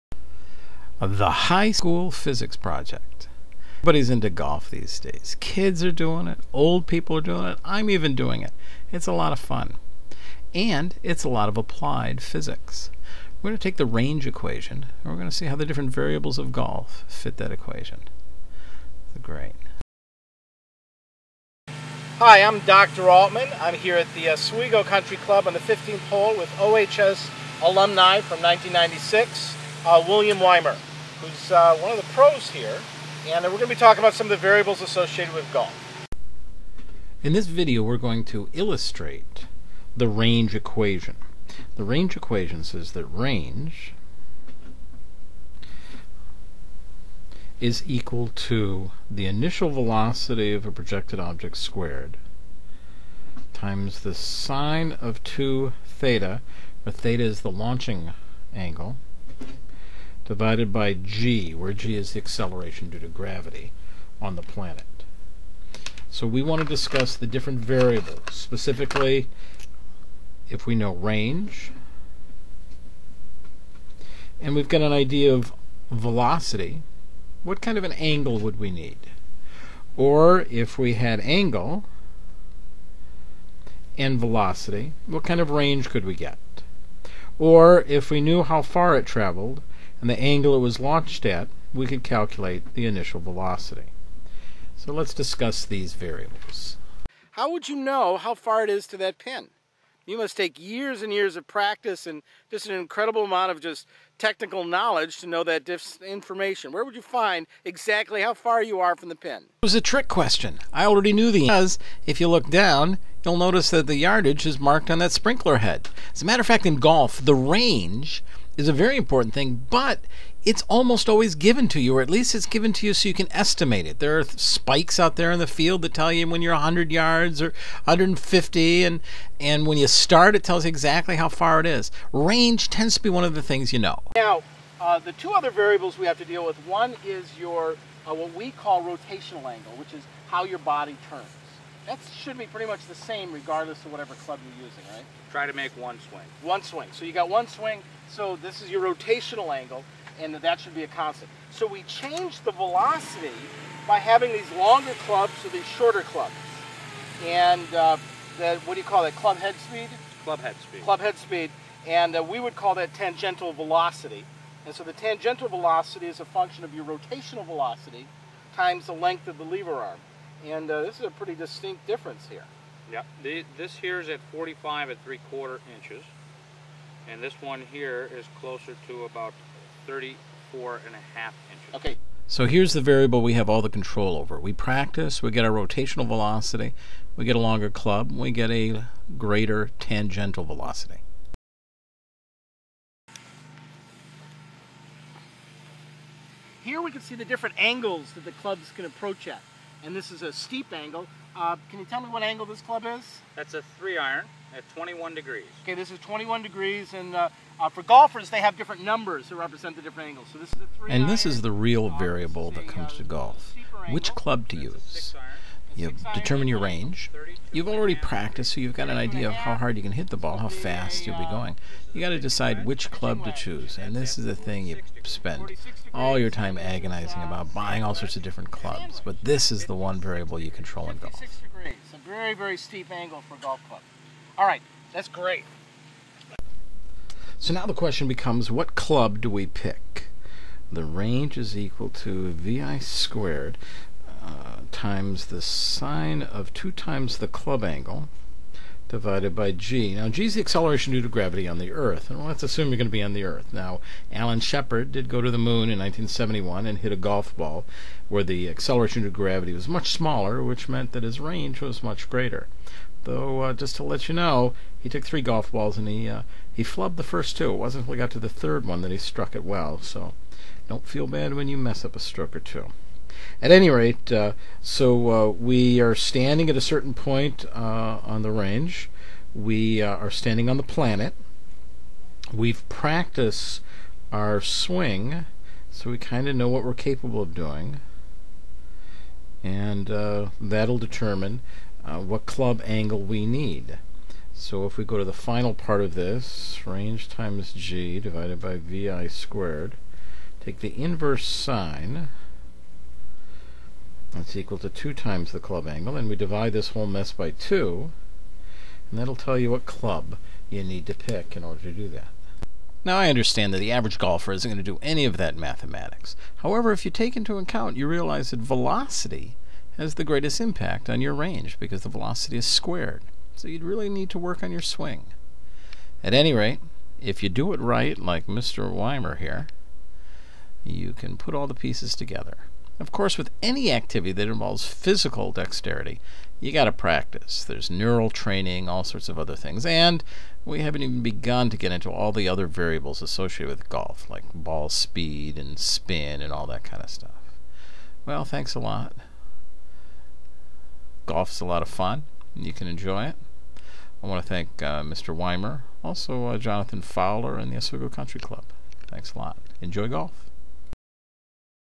Golf physics explained
The High School Physics Project works with William Z. Weimer, golf pro at the Oswego Country Club.  Explains the variables that go into club choice.